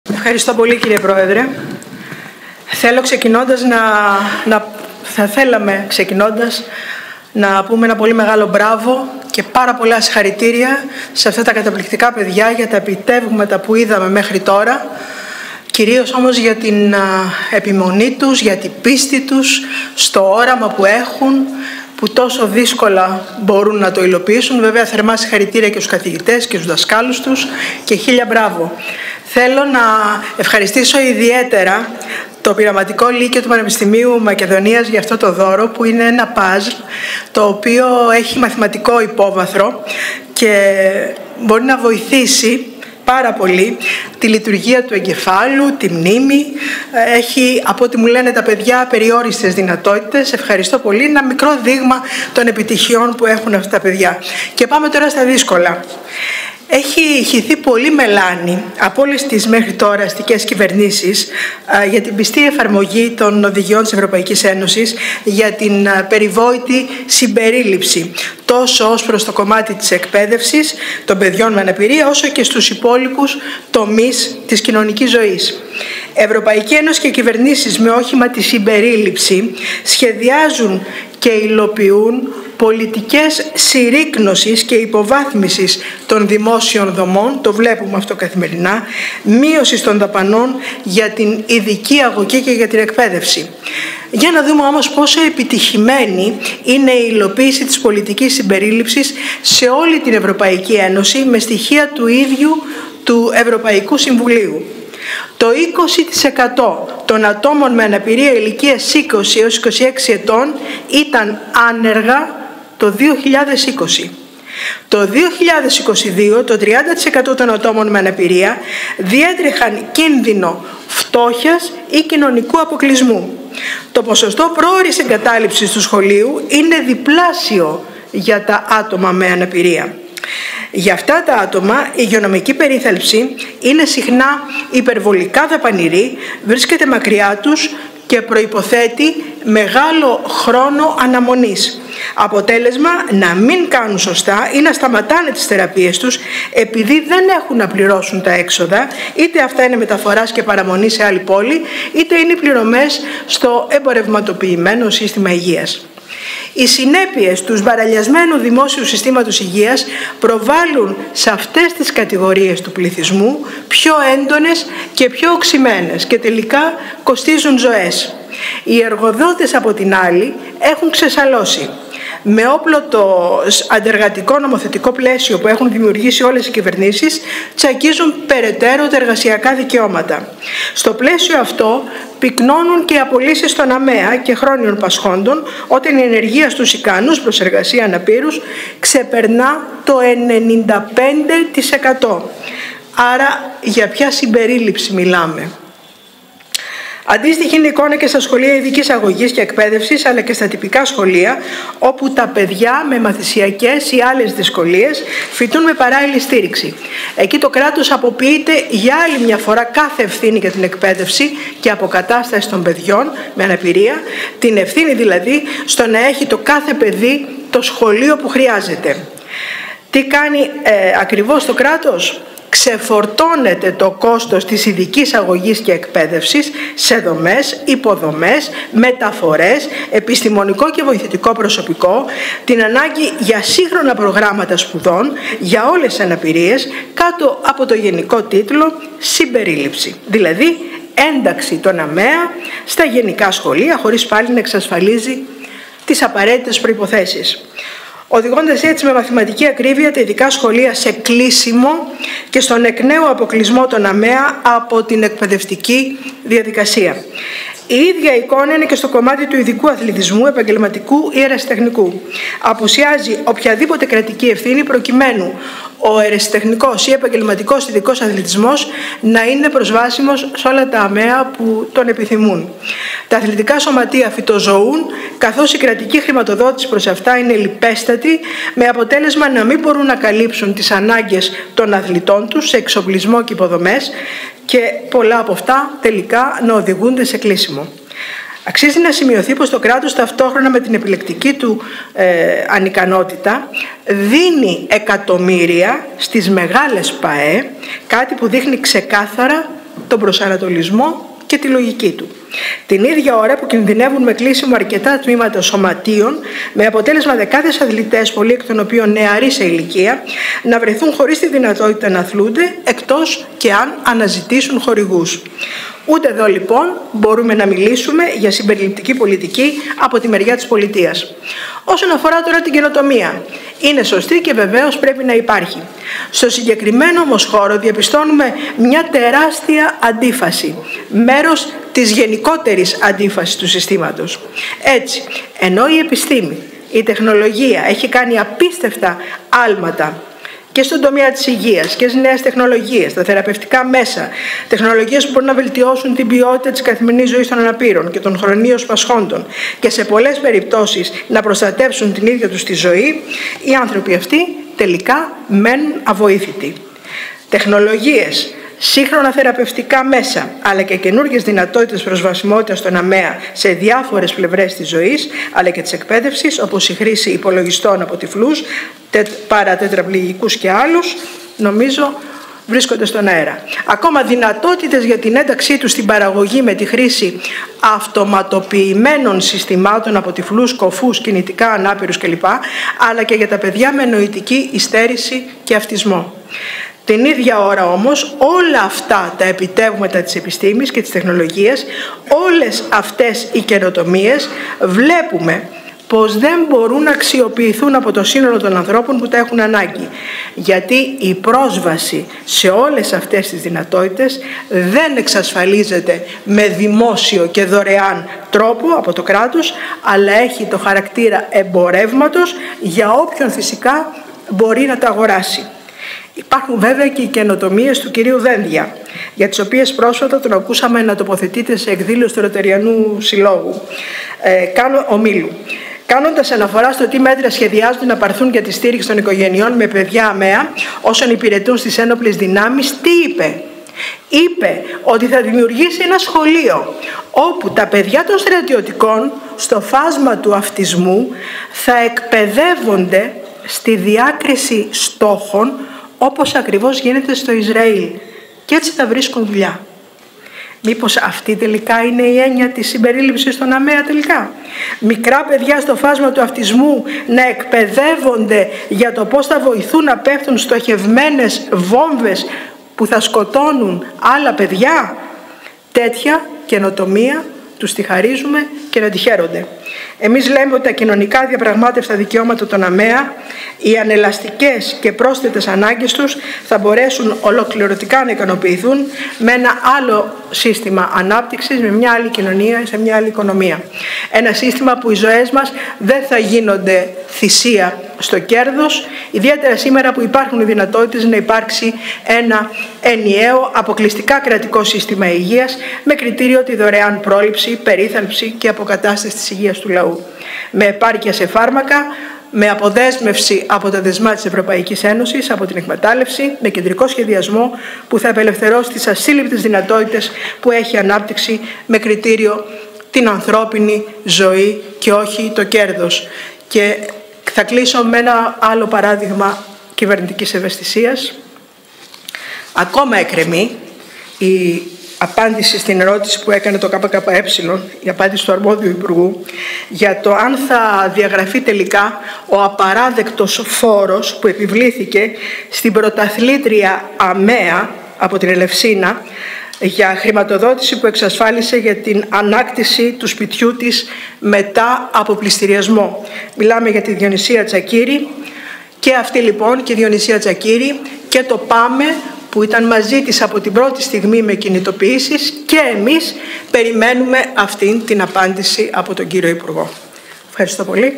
Σ. Διγενή: ΕΕ και κυβερνήσεις υλοποιούν πολιτικές μείωσης των δαπανών για την Ειδική Αγωγή
Η πρόσβαση των ατόμων με αναπηρία σε σύγχρονες επιστημονικές και τεχνολογικές δυνατότητες που μπορούν να τα διευκολύνουν στην καθημερινή τους ζωή «δεν εξασφαλίζεται με δημόσιο και δωρεάν τρόπο από το κράτος, αλλά έχει τον χαρακτήρα εμπορεύματος για όποιον φυσικά μπορεί να το αγοράσει», τόνισε η *Σεμίνα Διγενή,* βουλευτής του ΚΚΕ, μιλώντας σε κοινή συνεδρίαση Επιτροπών της Βουλής με θέμα την Καινοτομία και τη Συμπερίληψη.

Ανέδειξε, πιο συγκεκριμένα, την τεράστια αντίφαση του συστήματος, από τη μία πλευρά μέσα από τα απίστευτα άλματα της επιστήμης και της τεχνολογίας να μπορεί να βελτιωθεί ποιοτικά και να προστατευθεί η ζωή των αναπήρων και των χρόνιων πασχόντων, αλλά από την άλλη, τελικά να μένουν αβοήθητοι, καθώς τεχνολογίες και σύγχρονα θεραπευτικά μέσα «βρίσκονται στον αέρα».

Η Σ. Διγενή επισήμανε ότι υπάρχουν και κάποιες άλλες καινοτομίες, αυτές του κ. Δένδια, κάνοντας λόγο για τα όσα αισχρά δήλωσε σε ομιλία του στον Ροταριανό Όμιλο περί δημιουργίας σχολείων, ακολουθώντας σχετικό πρότυπο του ισραηλινού κράτους-δολοφόνου, όπου τα παιδιά των στρατιωτικών στο φάσμα του αυτισμού θα εκπαιδεύονται στη διάκριση στόχων και έτσι θα βρίσκουν δουλειά! Δηλαδή η κυβέρνηση οραματίζεται, όπως είπε χαρακτηριστικά και η βουλευτής του ΚΚΕ, «μικρά παιδιά στο φάσμα του αυτισμού να εκπαιδεύονται για το πώς θα βοηθούν να πέφτουν στοχευμένες βόμβες που θα σκοτώνουν άλλα παιδιά. Τέτοια καινοτομία τους την χαρίζουμε και να την χαίρονται».

Υπενθύμισε ότι Ευρωπαϊκή Ένωση και κυβερνήσεις «με όχημα τη συμπερίληψη σχεδιάζουν και υλοποιούν πολιτικές συρρίκνωσης και υποβάθμισης των δημόσιων δομών, μείωσης των δαπανών για την Ειδική Αγωγή και Εκπαίδευση». Όπως εξήγησε, στο πλαίσιο του αντεργατικό νομοθετικό πλαισίου προς όφελος της εργοδοσίας πυκνώνουν απολύσεις ΑμεΑ ή χρόνιων πασχόντων, ενώ η υγειονομική τους περίθαλψη είναι δαπανηρή, βρίσκεται μακρυά τους και προϋποθέτει μεγάλο χρόνο αναμονής, «με αποτέλεσμα να μην κάνουν σωστά ή να σταματούν τις θεραπείες τους» είτε επειδή δεν έχουν να πληρώσουν τα έξοδα παραμονής και μεταφοράς σε άλλη πόλη, είτε επειδή καταφεύγουν και πληρώνουν στο εμπορευματοποιημένο σύστημα Υγείας: «Οι συνέπειες του σμπαραλιασμένου συστήματος Υγείας προβάλλουν σε αυτές τις κατηγορίες του πληθυσμού πιο έντονες και πιο οξυμένες και τελικά κοστίζουν ζωές».